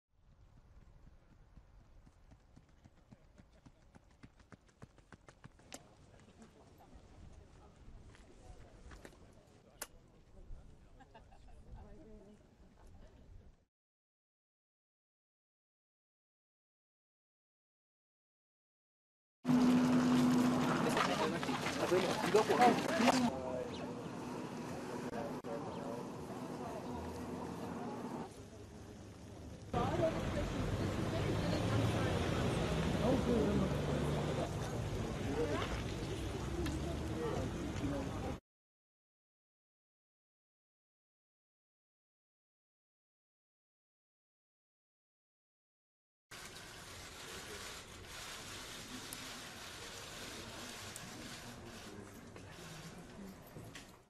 Oscar Pistorius under house arrest
Double-amputee Olympic runner Oscar Pistorius was released from jail on Monday and placed under house arrest. Pistorius fatally shot his girlfriend in 2013